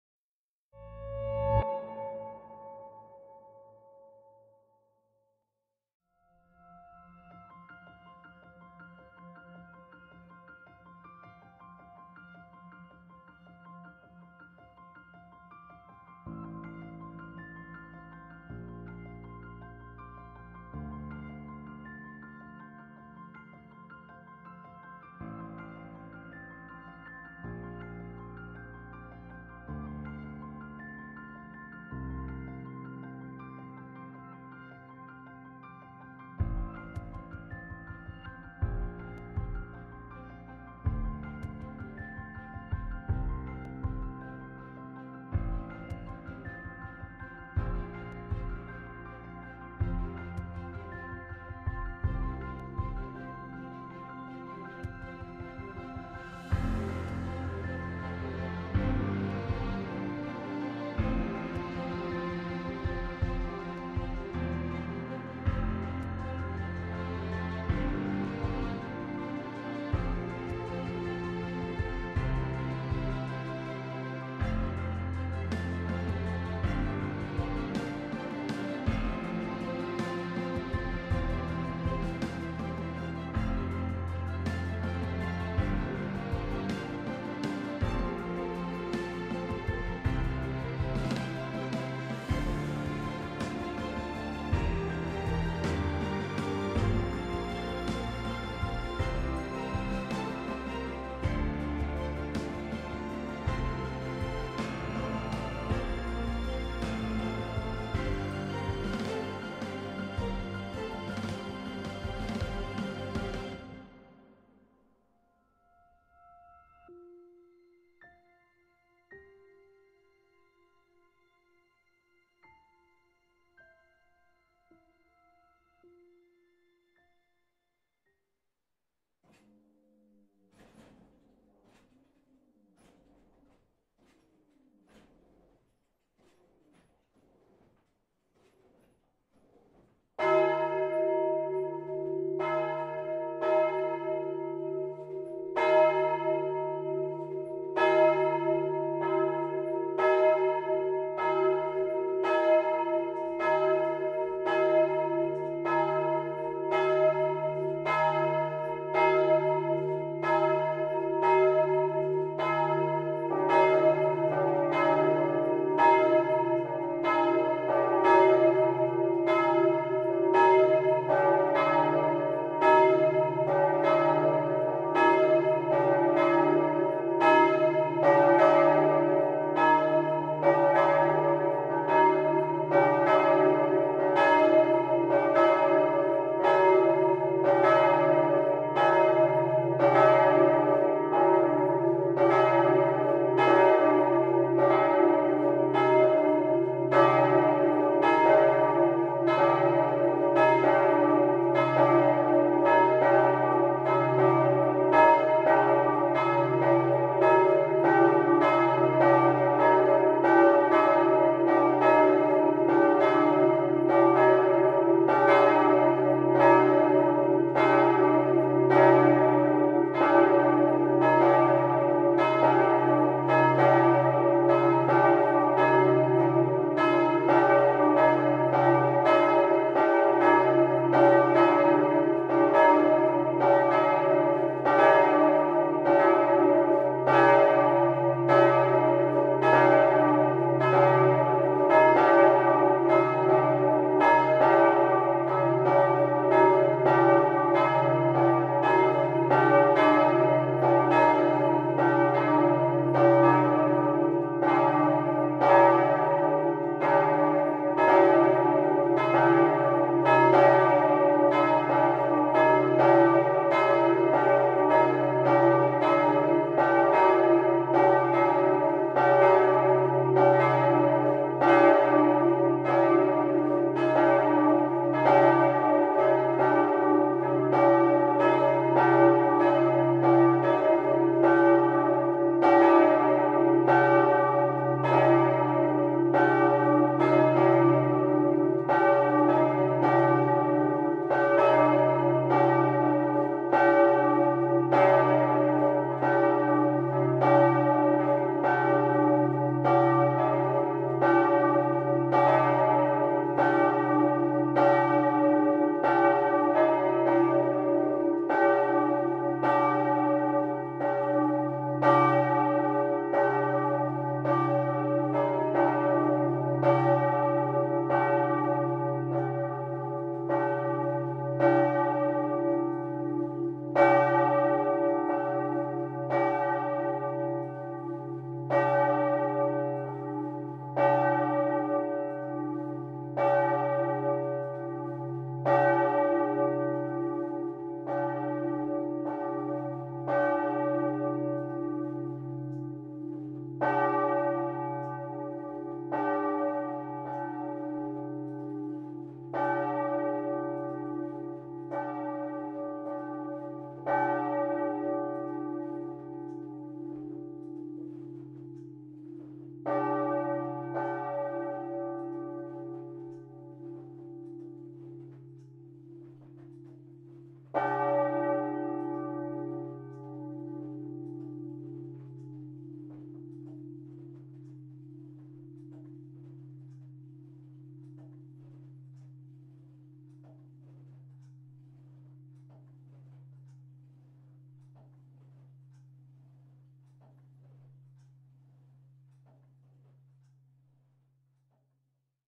Cloches: Sainte-Colombe (25 300), Eglise Sainte-Colombe
Cloche 1: « Marie-Colombe-Françoise », Diamètre 135,4 cm, Poids 1 500 kg, Fondue par J-B Beau, En 1 846, à Pontarlier, Chante le Ré3.
Cloche 2: « Euphrasie-Éléonore-Julie », Diamètre 116,5 cm, Poids 855 kg, Fondue par F-J Bournez, En 1 868, à Morteau, Chante le Mib3.

Début de la sonnerie à 2:09

Je remercie comme il se doit M. Malfroy, maire de cette charmante commune, pour son autorisation que de monter au clocher, et pour l’ouverture de ce dernier, je le remercie de-même pour la sonnerie des cloches spécialement pour la vidéo.

Je remercie également M. Claudet, 1er Adjoint au maire, pour l’ouverture de la sacristie.